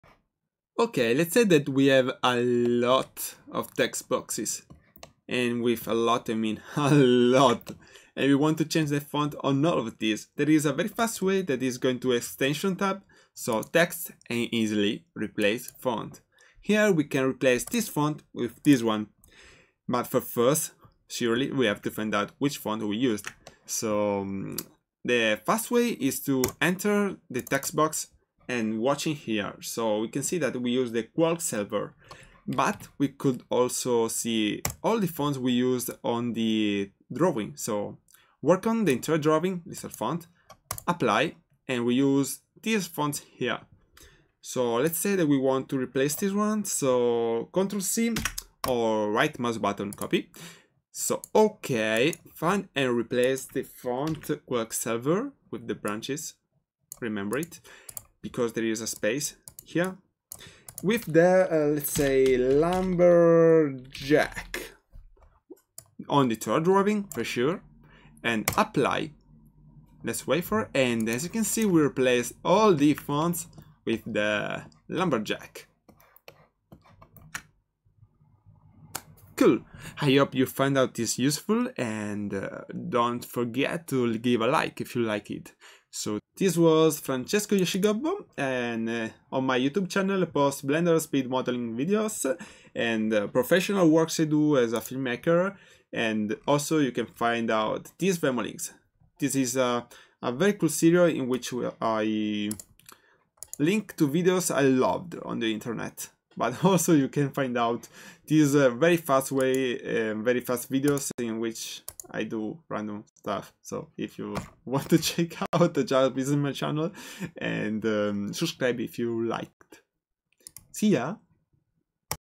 「 how to 」 replace all the fonts in once - Inkscape tutorial
If You have A LOT of fonts to replace, well.. that is the moment the headache starts !!
But following this simple guide You'll end up with a lot of unspent time ^^
I found myself into this situation, I solved so I hope this will solve your one too :)

☆★☆★ ABOUT ME ☆★☆★

You can also find me on your favorite social network serachin' for "FraYoshi"